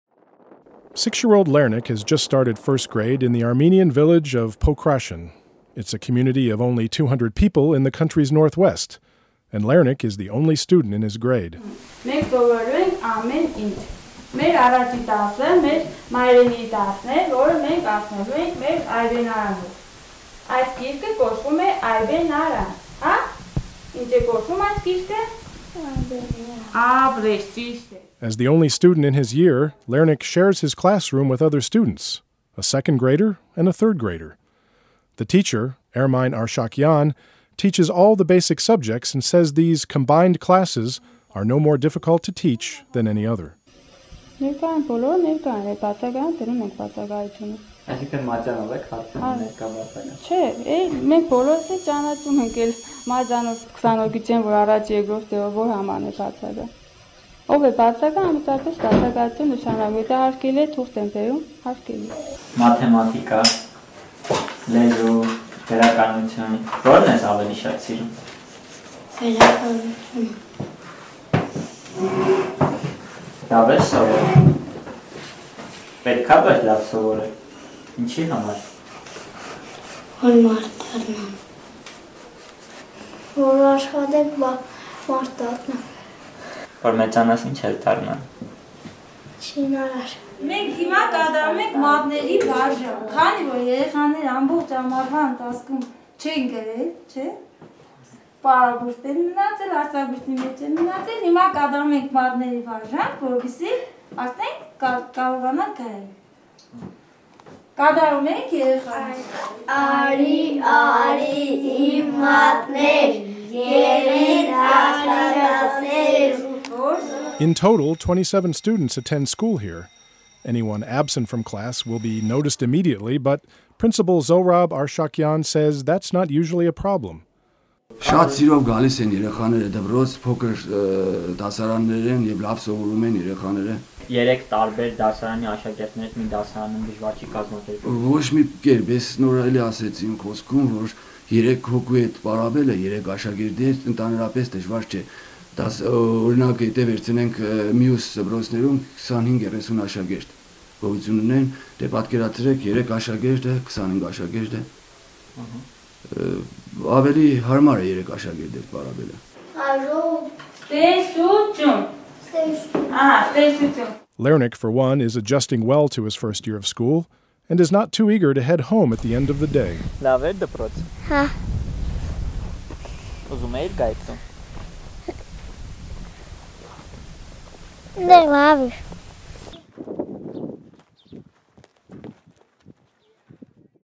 Starting School In Armenia, In A Class Of One
The school year has begun, and kids everywhere are readjusting to classes and homework. But in the tiny Armenian village of Pokrashen, one six-year-old, Lernik, has to get used to attending first grade all by himself.